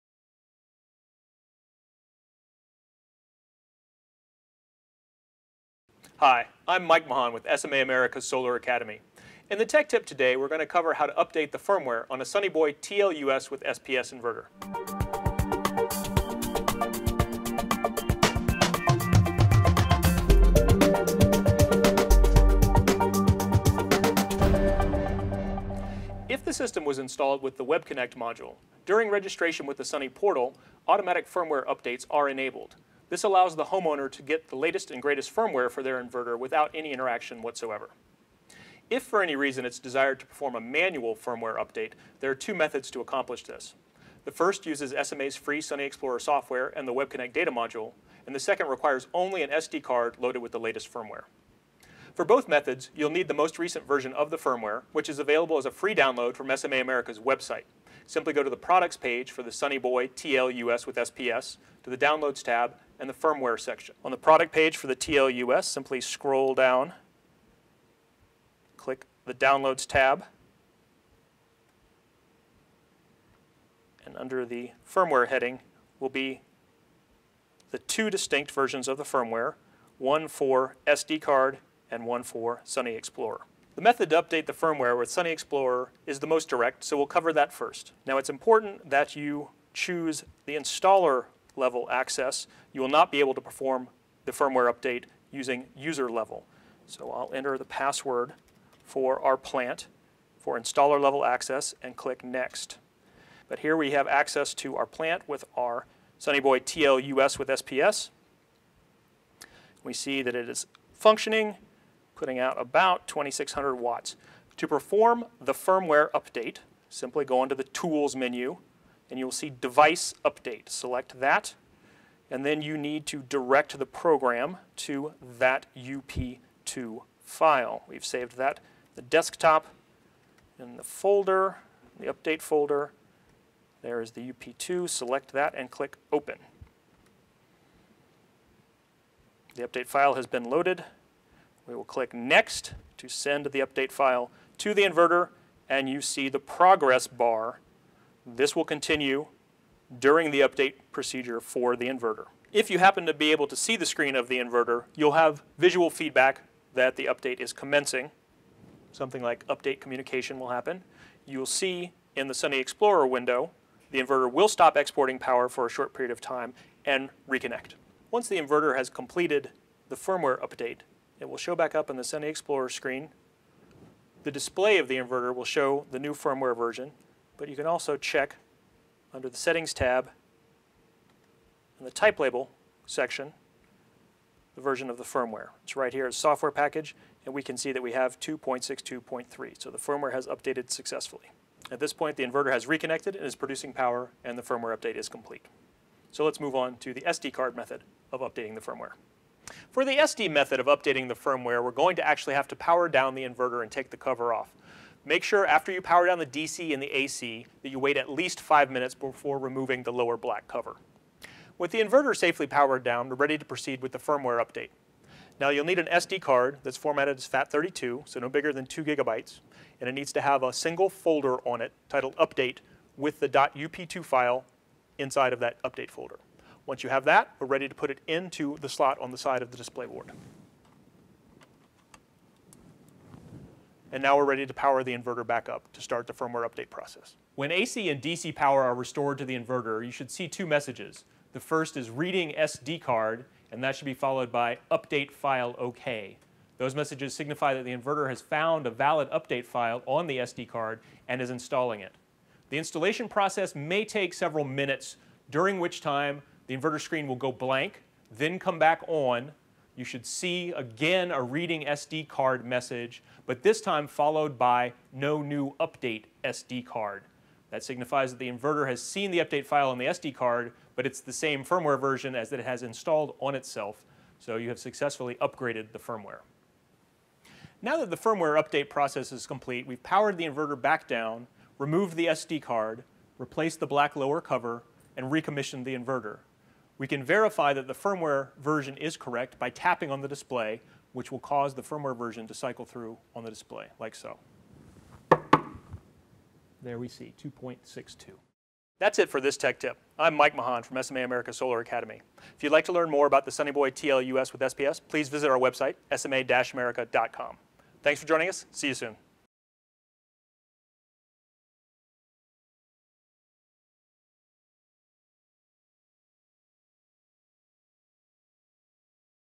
Tech Tip: Sunny Boy TL-US Firmware Update
Updating a Sunny Boy TL-US' firmware ensures optimal operation with the latest features. The simple process can be completed automatically or manually with a few easy steps.